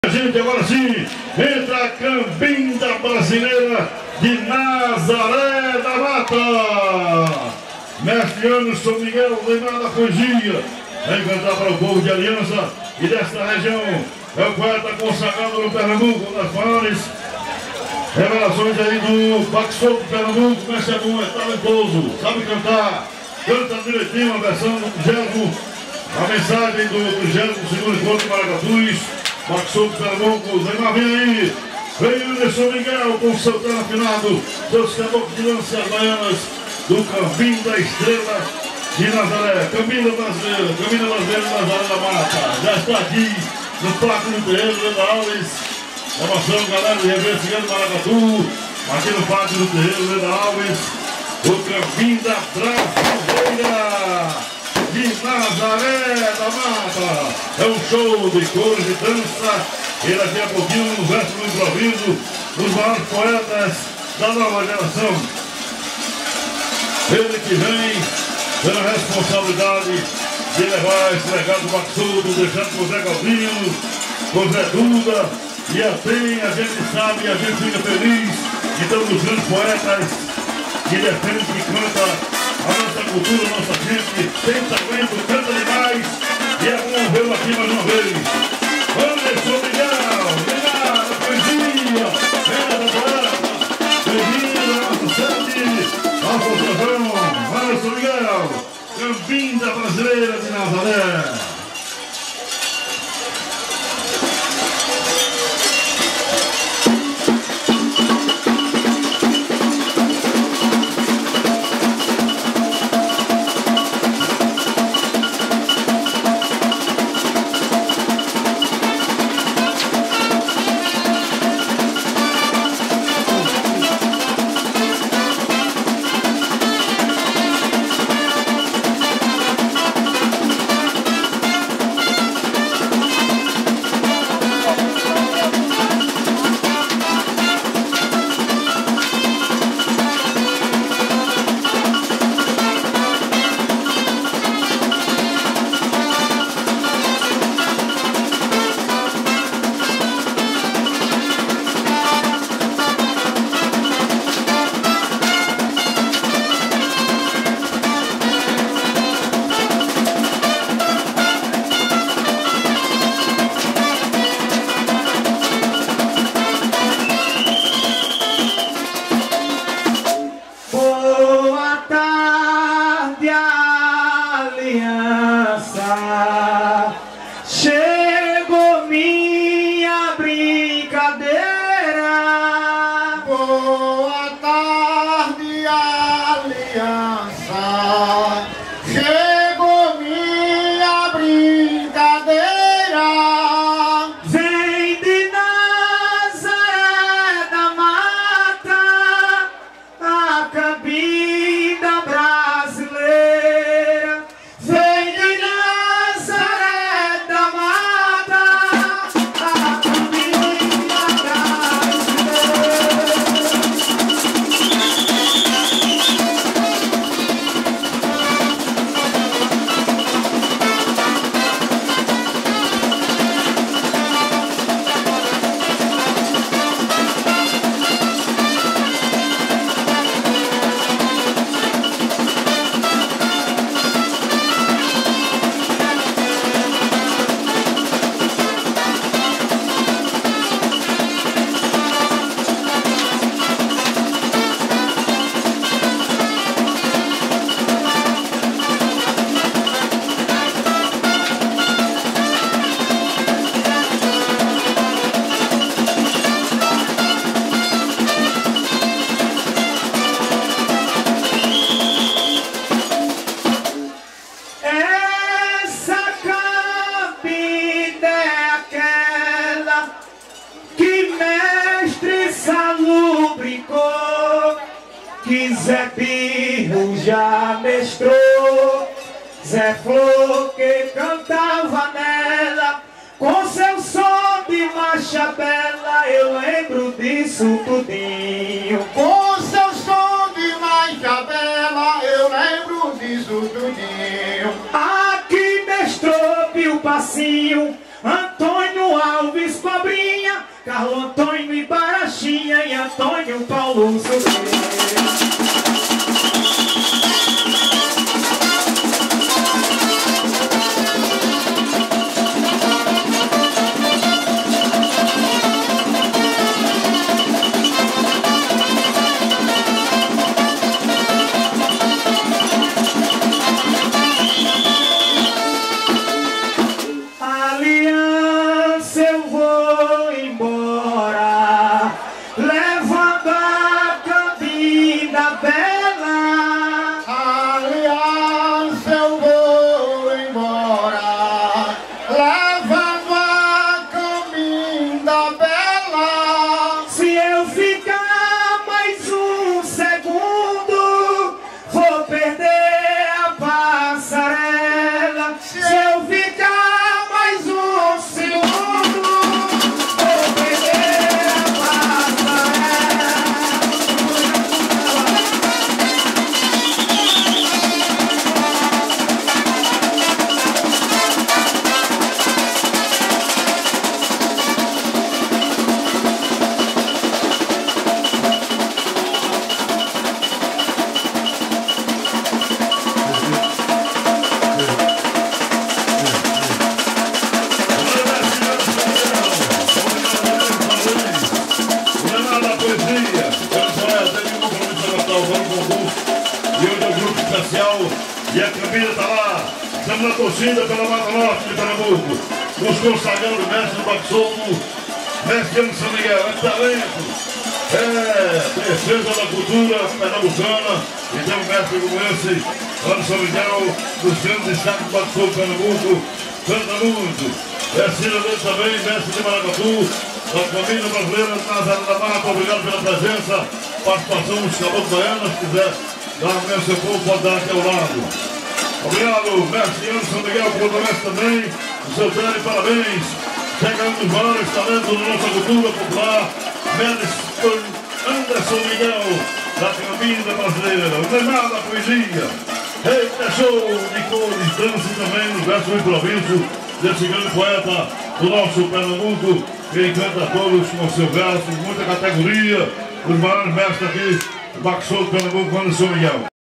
🐟🇧🇷 Cambinda Brasileira na Associação de Maracatus de Baque Solto Carnaval 2024 🎊✨️
Breve apresentação da Cambinda Brasileira na terça-feira de Carnaval 2024 na Associação de Maracatus em Aliança/PE, sob os apitos do Mestre Anderson Miguel e Contramestre Davi Coutinho.

Na ocasião, a Caboclaria, assim como a de outros grupos, não estava usando os chapéus devido às chuvas  que aconteceram na noite anterior.